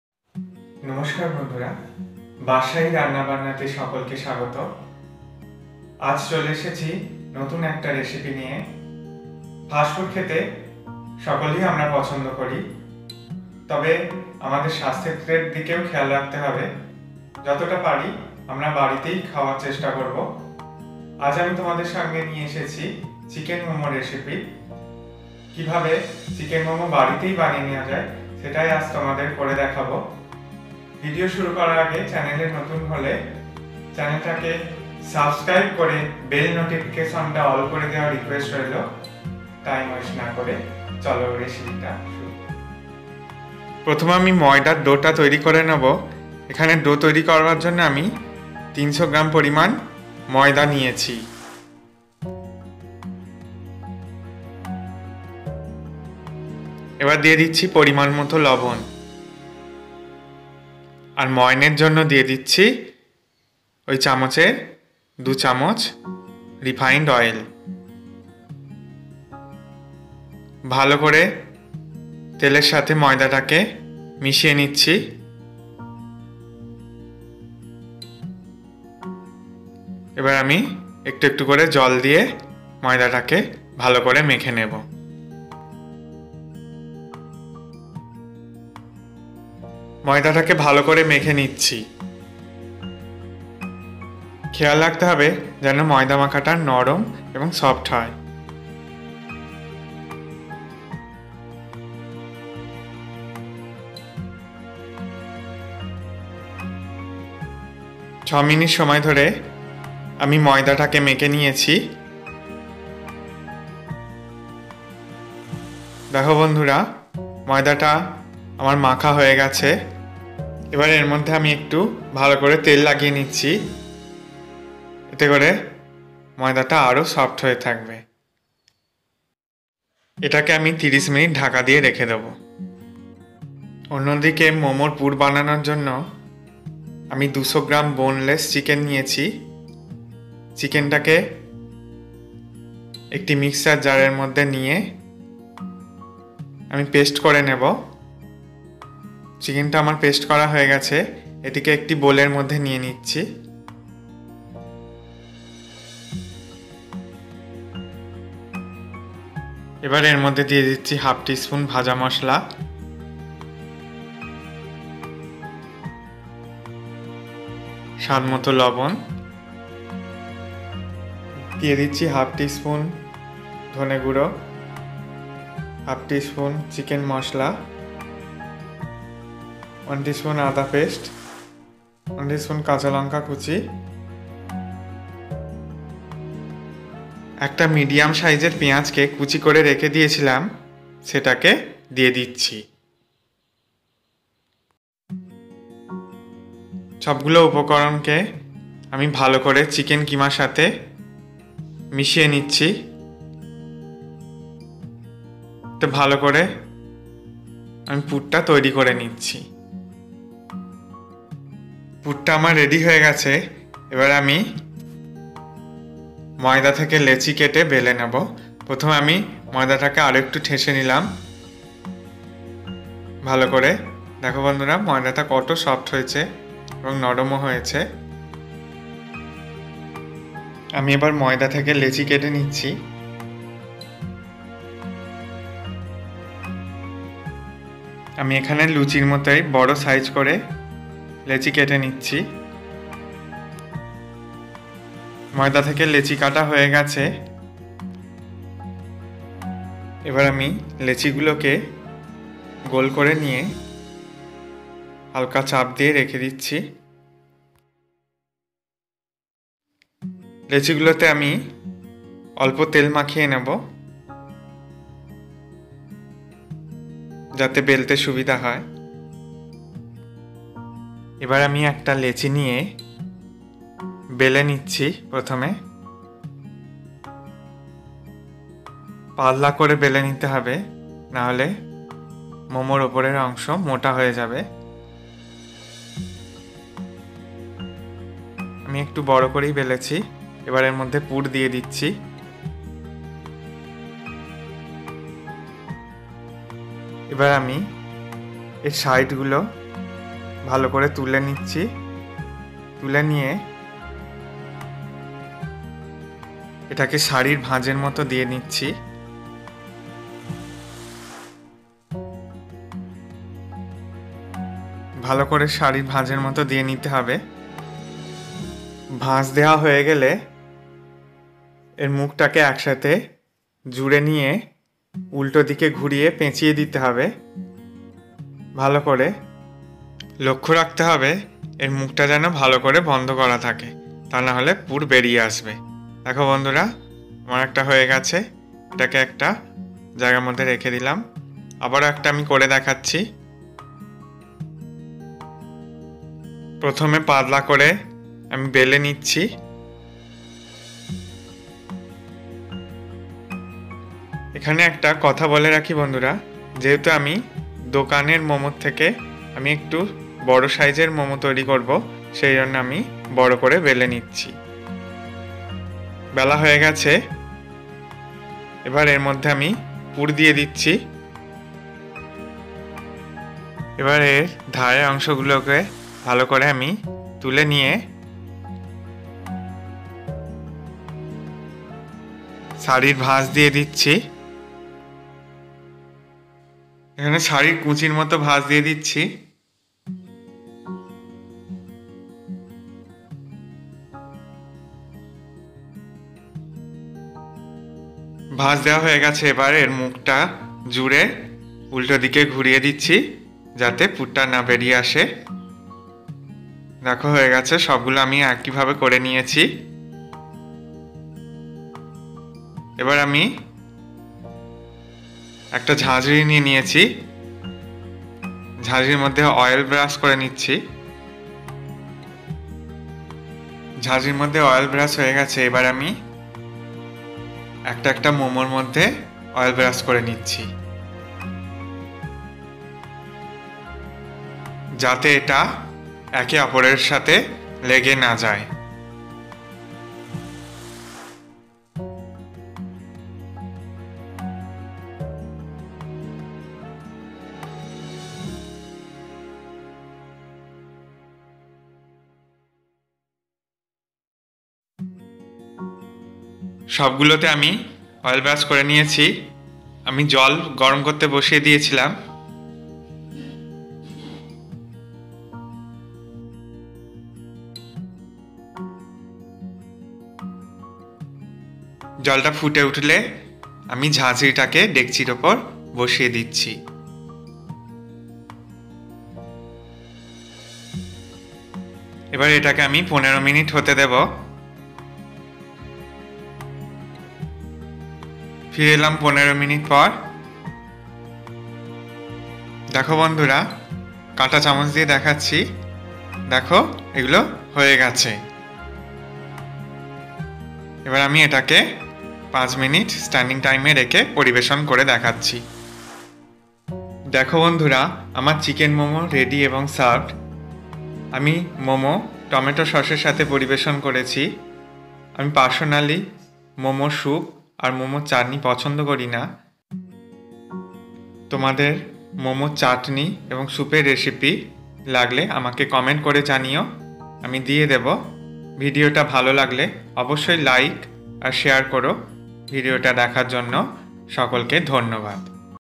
বাড়িতে সহজেই দোকানের মতো চিকেন মোমো তৈরির রেসিপি।।chicken momo recipe in bengali।।চিকেন মোমো রেসিপি।
chicken momo recipe---

Ingredients---

Flour-300gm
Salt as per taste
Refined oil-2tsp
Boneless chicken-200gm
Fry masala-1/2tsp cumin seed coriander seed & red chilli
Chicken masala-1/2tsp
Coriander powder-1/২tsp
Ginger paste-1tsp
Garlic paste-1tsp
Chopped green chilli-2tsp
Chopped onion-1nos medium size
Water for boiled